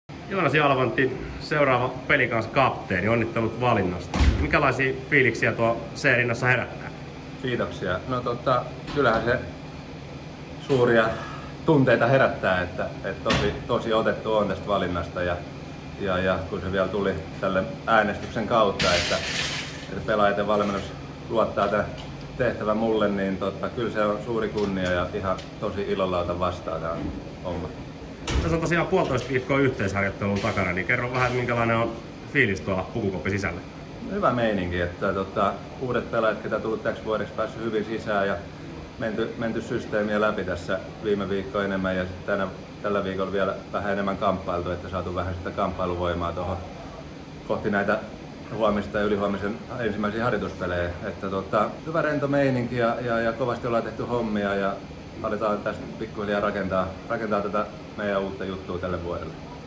Joonas Jalvanti, seuraava Pelicans-kapteeni
Joonas Jalvanti kantaa C-kirjainta rinnassa kauden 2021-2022. Kipparin kommentit valinnasta kapteeniksi. 👊

🗣 "Suuria tunteita se tietenkin herättää"